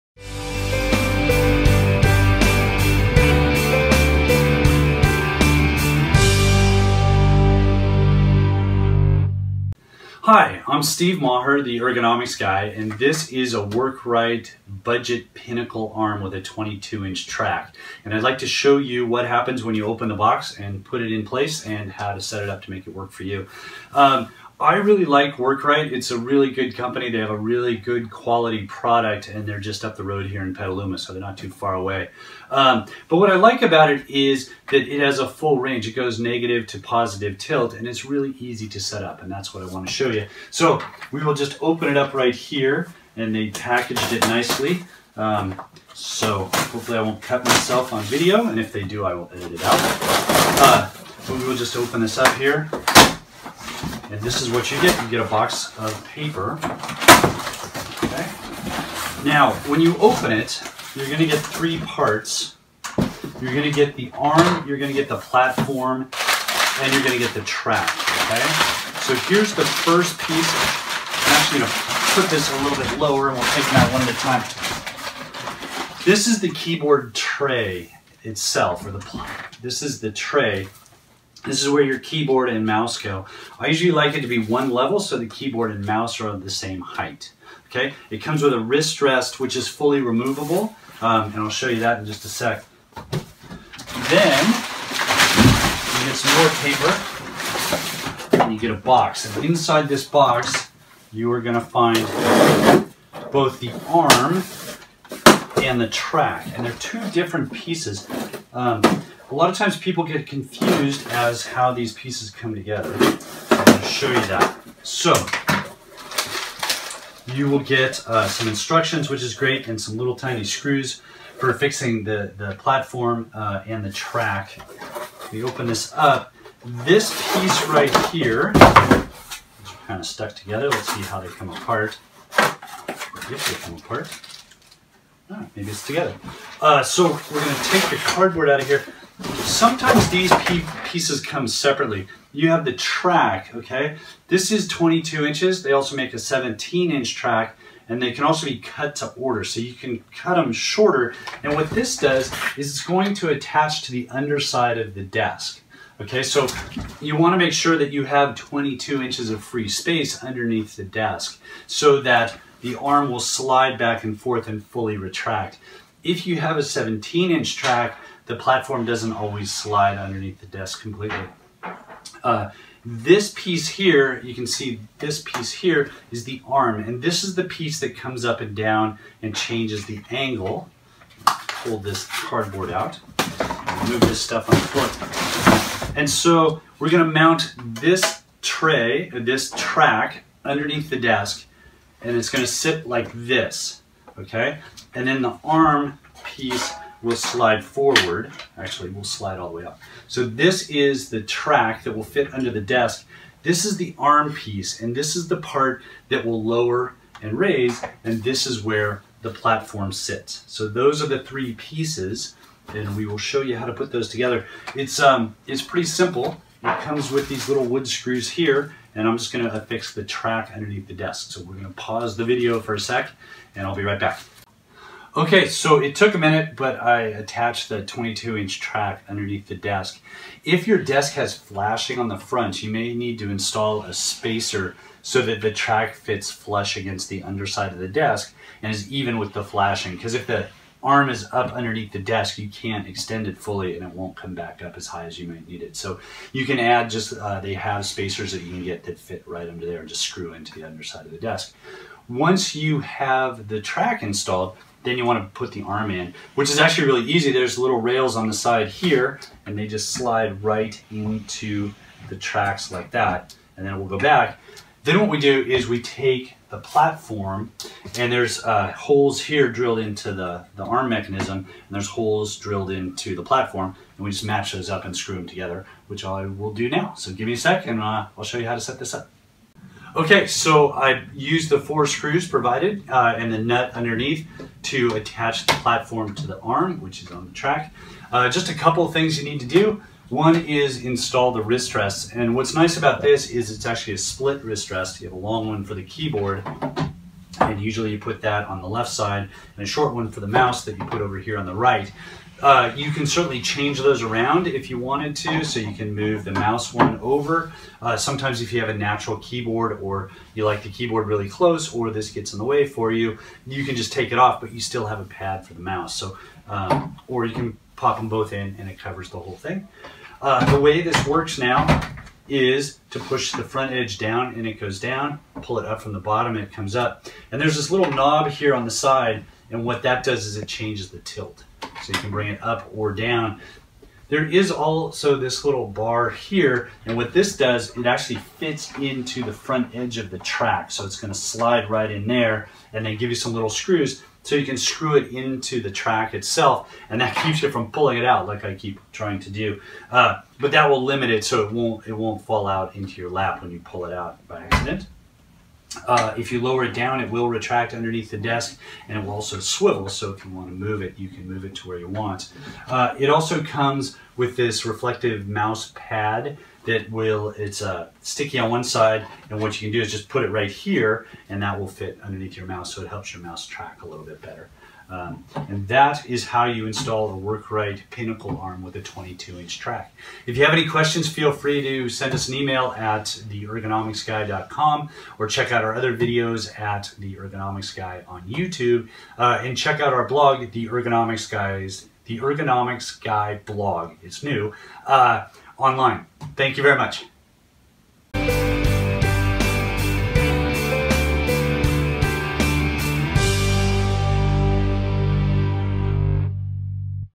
Worrkrite Pinnacle keyboard tray
How to install a Workrite Pinnacle keyboard tray. The keyboard tray system  is composed of the track, arm and platform. Be sure to use eye protection when working under the desk!.